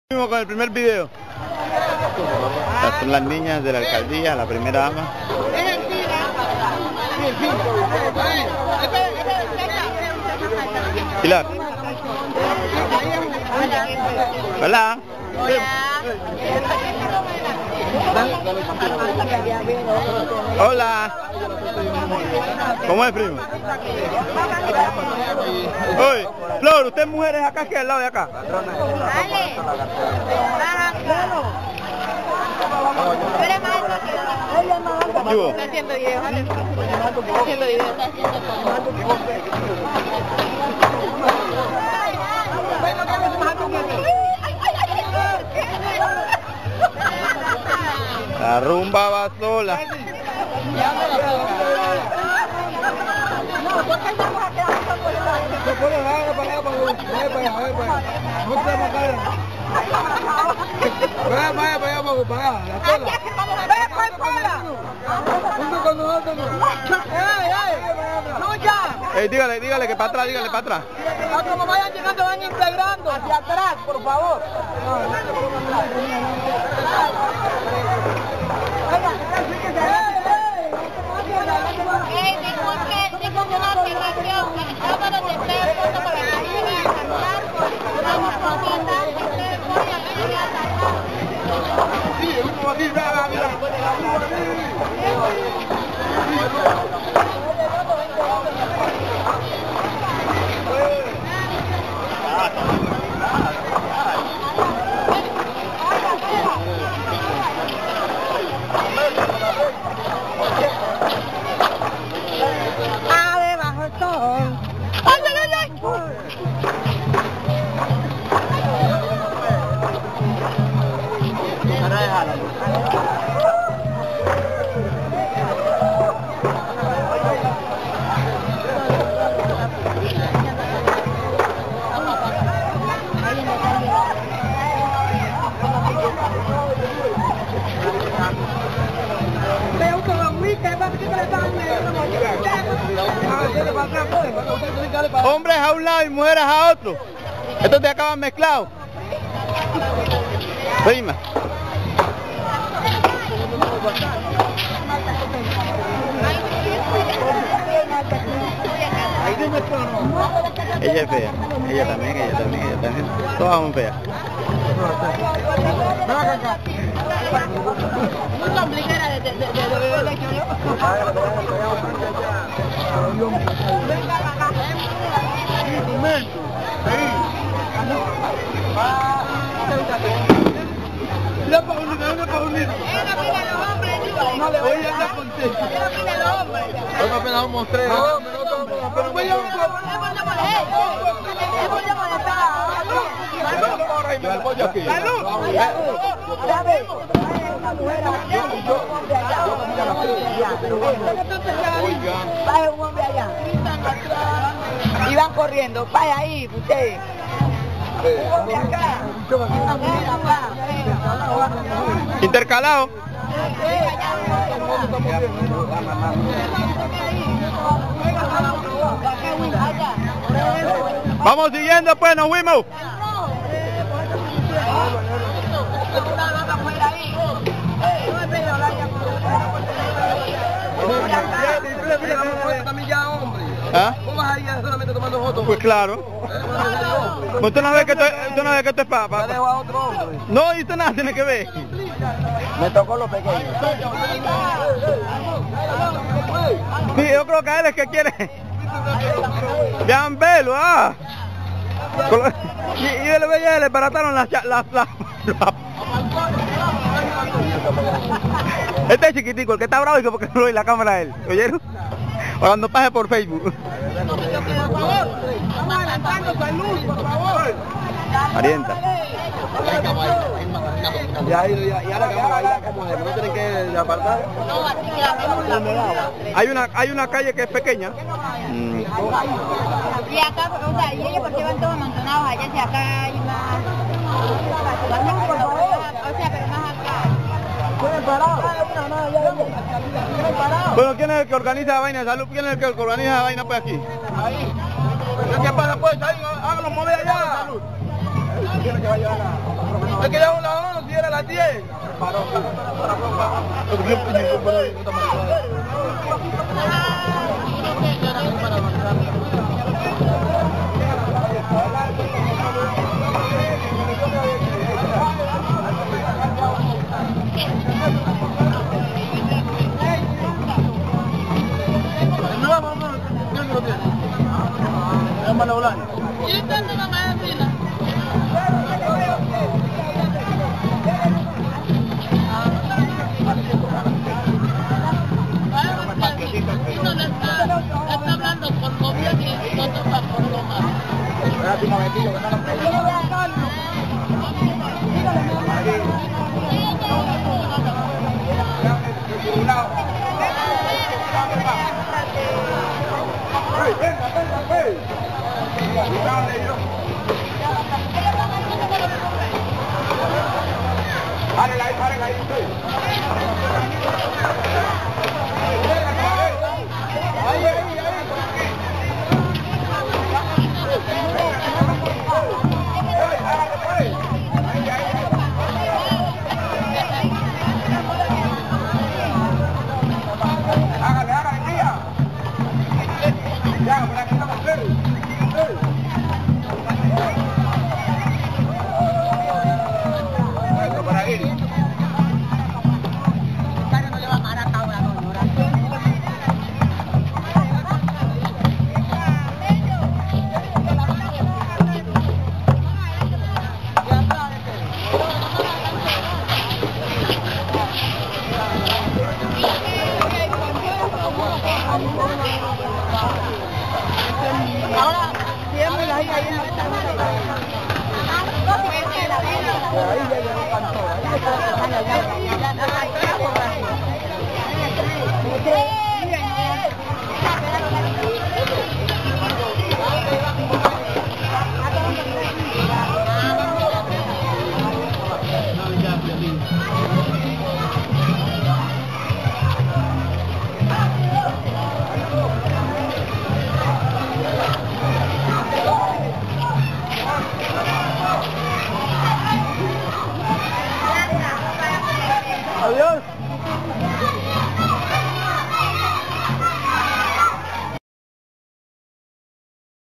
Fiestas patronales la tola
TITULO: Fiestas patronales la tola
SINOPSIS: Desfile del 23 de octubre con la participación de las entidades del municipio La Tola en el departamento de Nariño territorio del Pacifico Sur Colombiano.
SERIE: Prácticas Tradicionales
AUTOR: Alexander Ordoñez
DURACIÓN: 00:7:39
AUTOR: Municipio La Tola/Nariño
REALIZADOR: Alexander Ordoñez
FECHA: 25 de noviembre de 2012